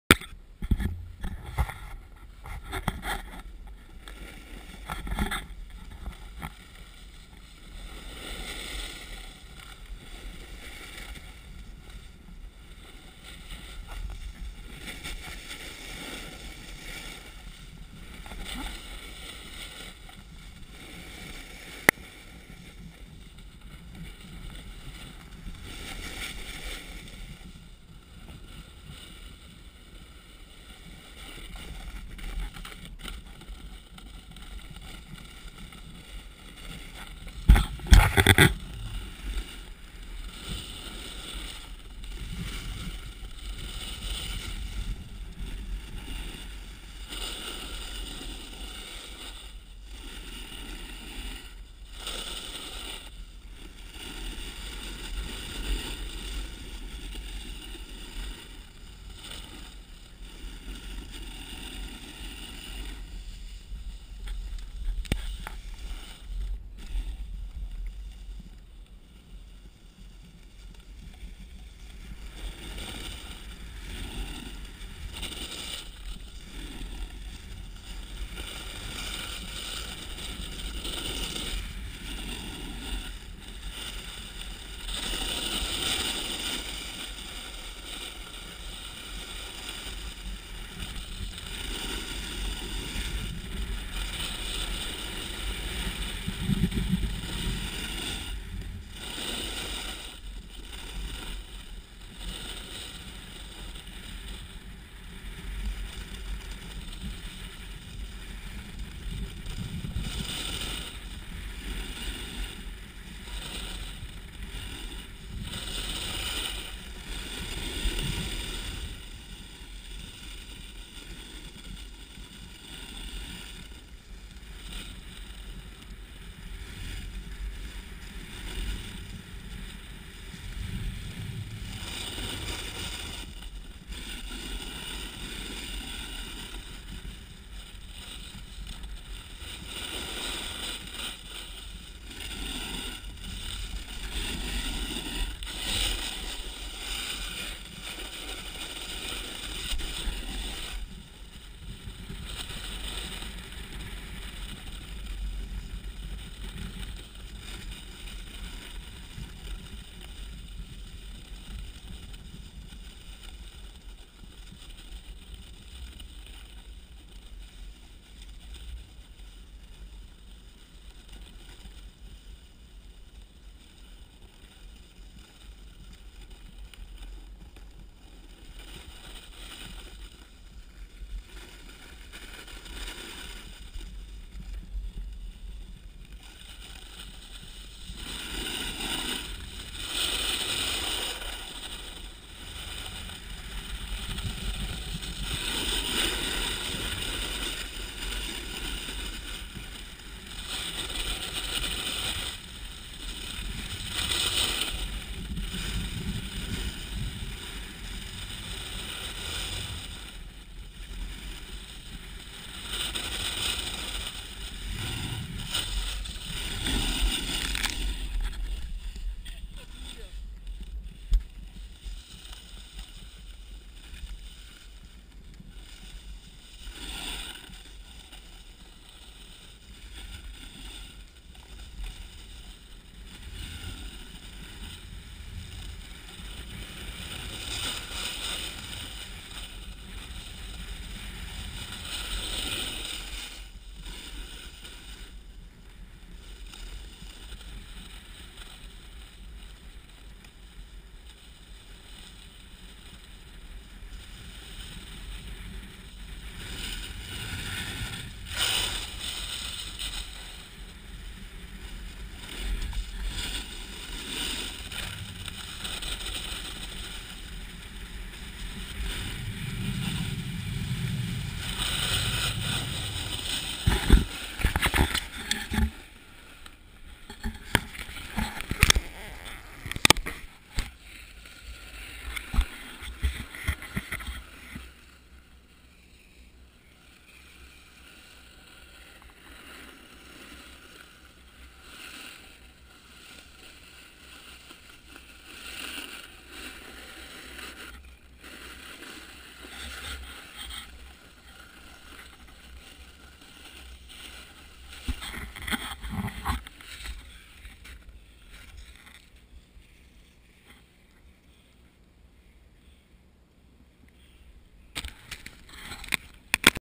Valtournenche snowboarding xsories gopro
Snowboarding in Valtournenche with GoPro and U-SHOT by XSORIES